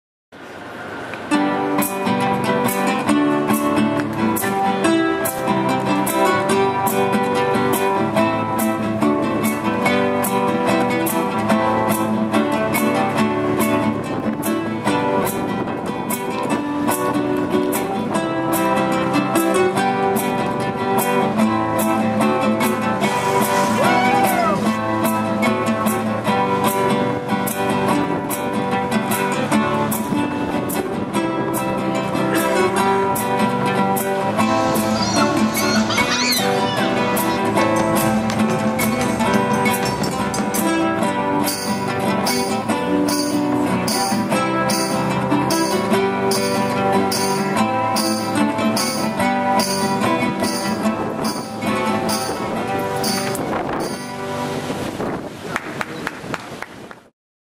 ASHEVILLE
Asheville, NC. Music by The Buskits